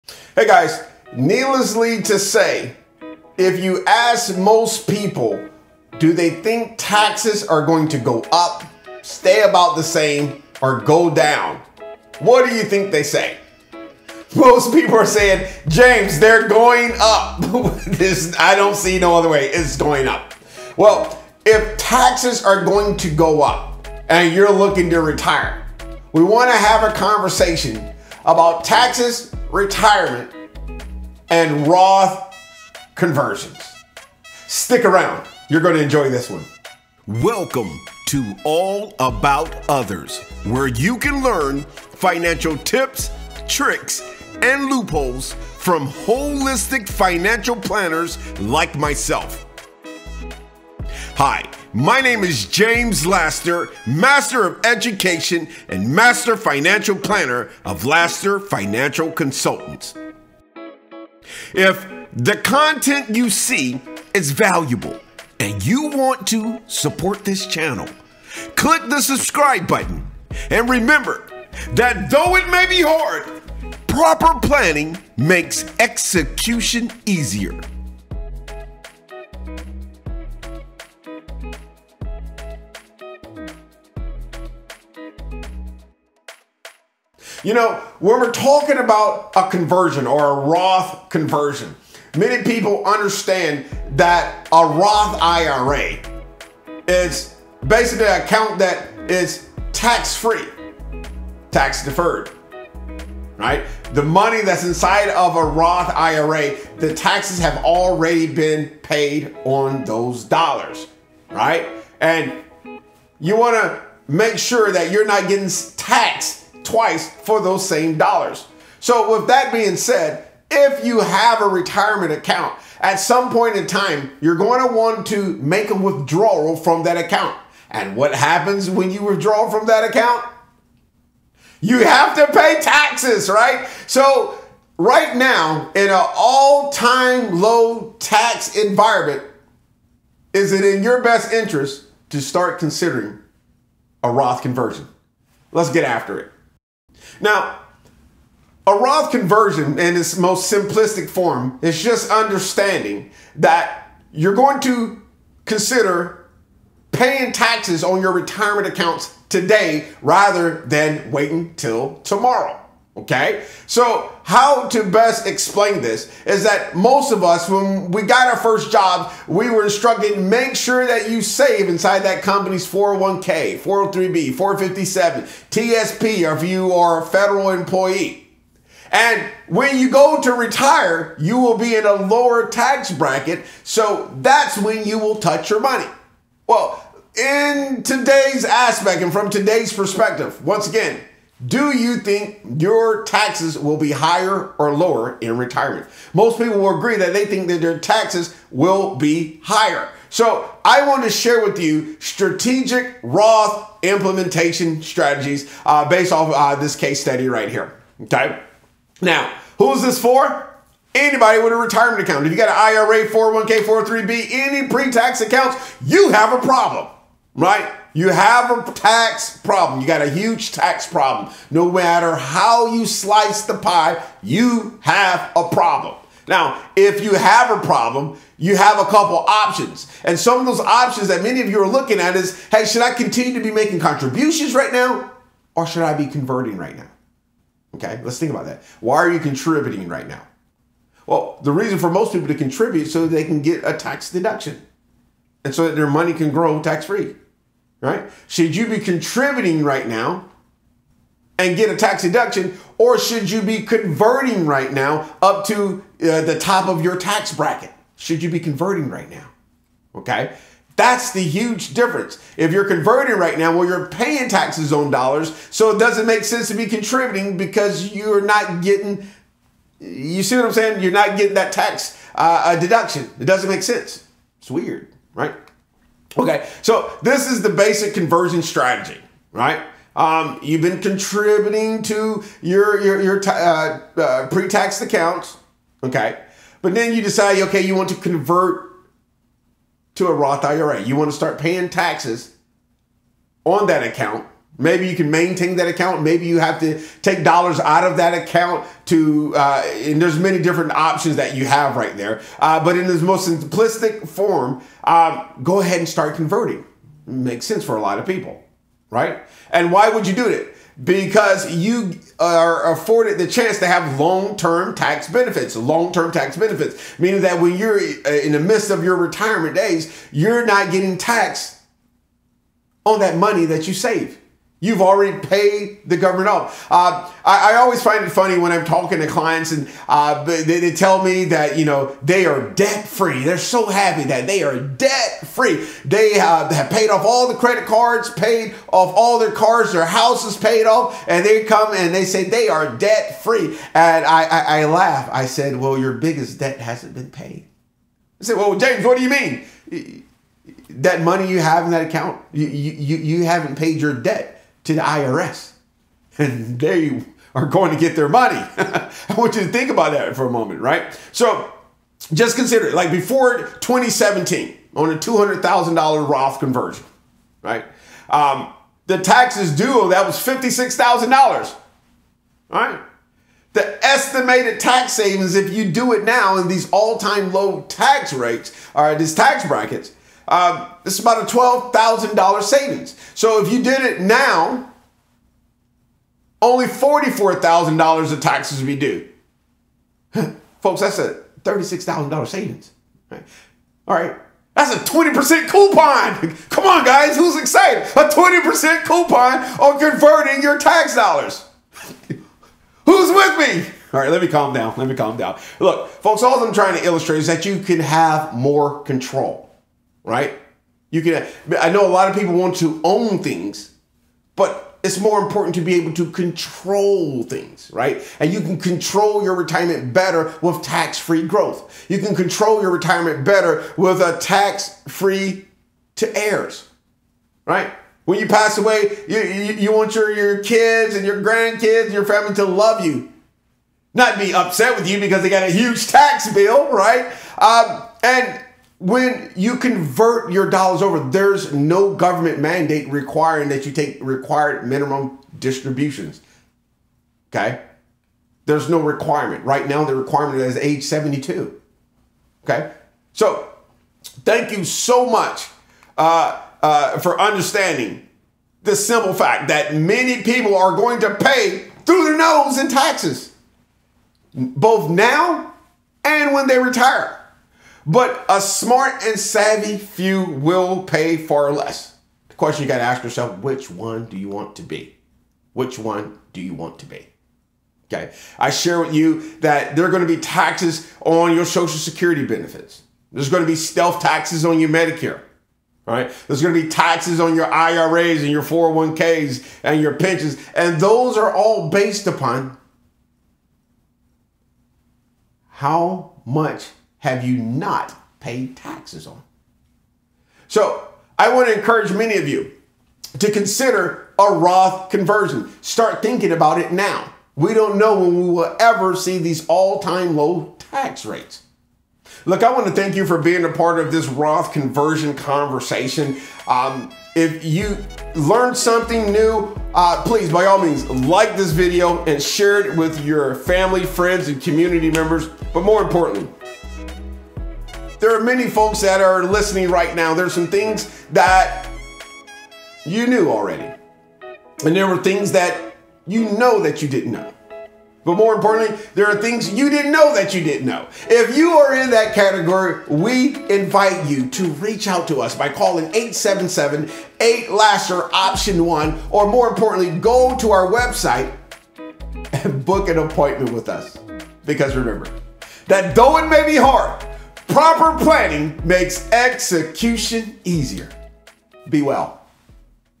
Roth Conversion Implementation
In this video, James discusses Roth Conversion Implementation. If you would like a one on one consultation please head over Also, come join us on our Facebook